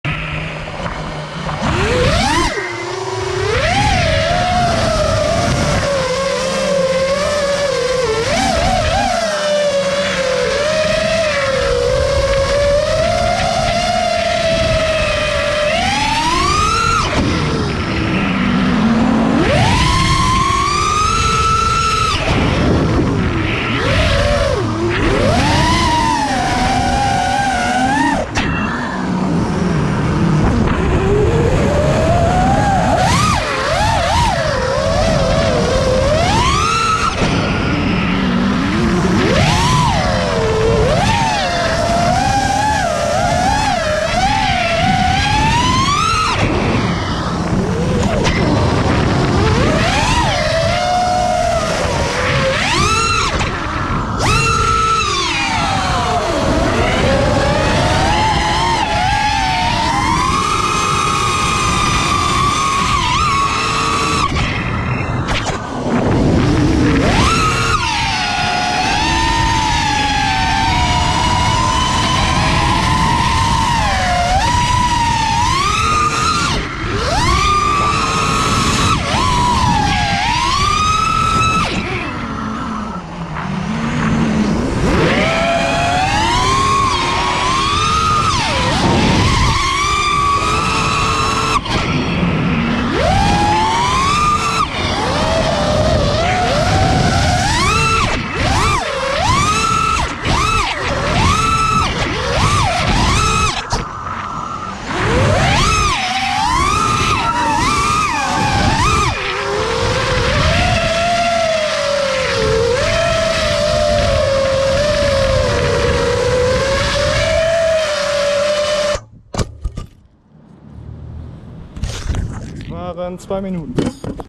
How to drain LiPo batteries fast 🔋🚀
Mate: Hurry up, I need to get to work (already packing bag)

I used a ND4 on a Hero 7 black. Unfortunately missed to lower shutter speed - had to mask the over exposure using Resolves color grading. Is it too colorful?